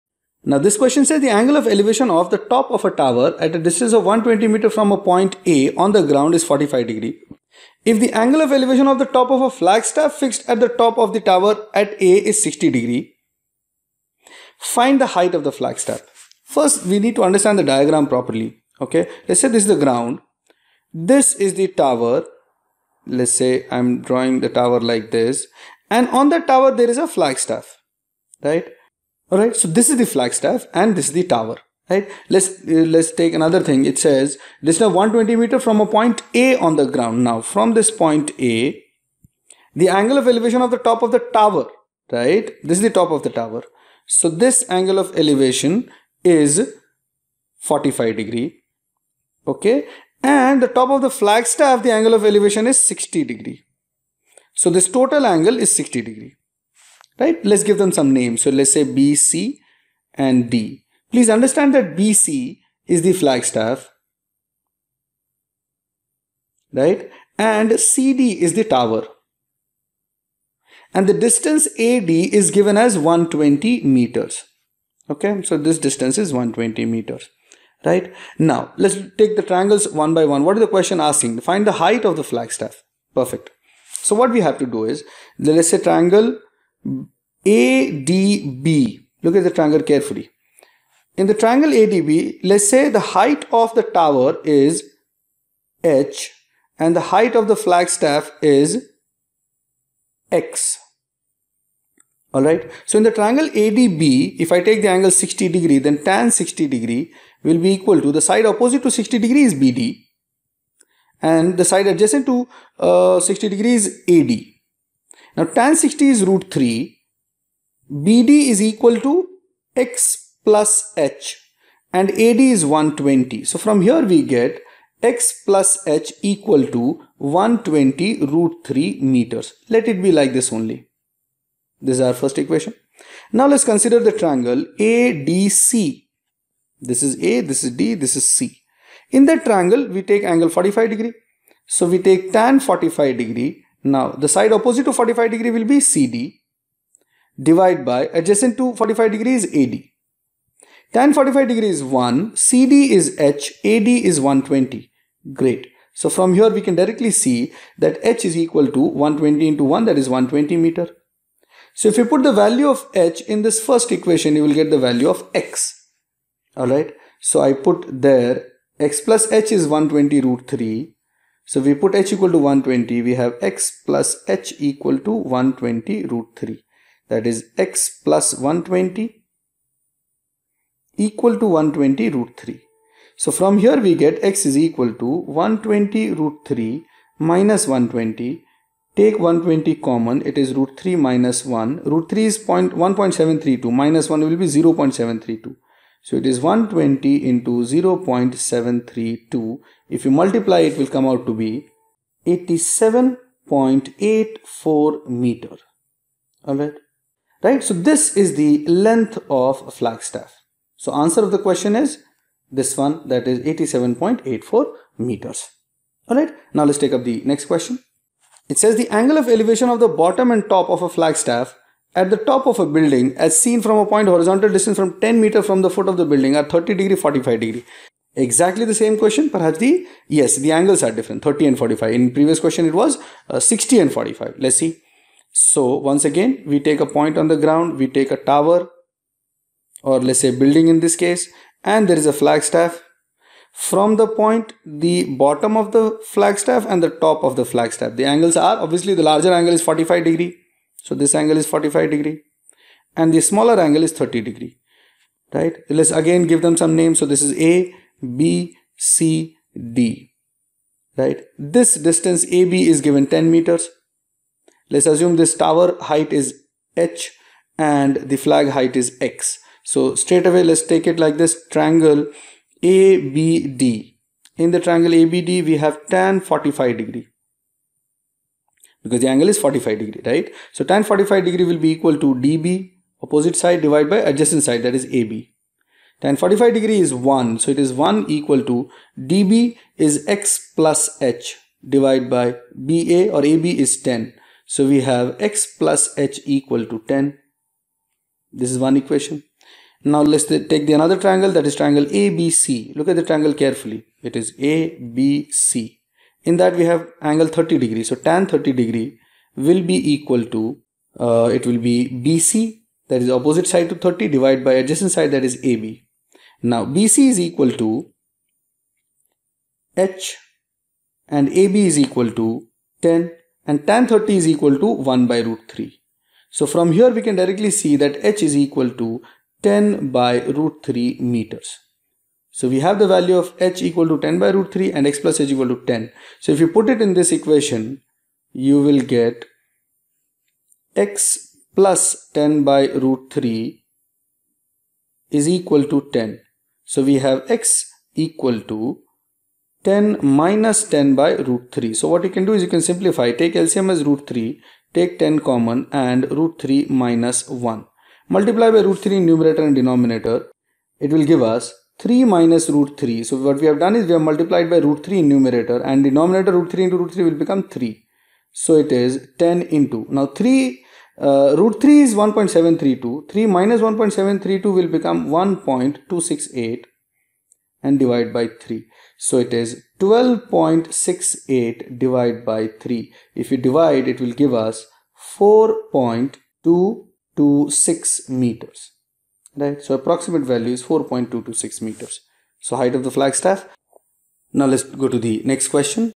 Flagstaff Questions in Height and Distance
This video includes the explanation of some good questions in Height and Distance related to Height of a Flagstaff.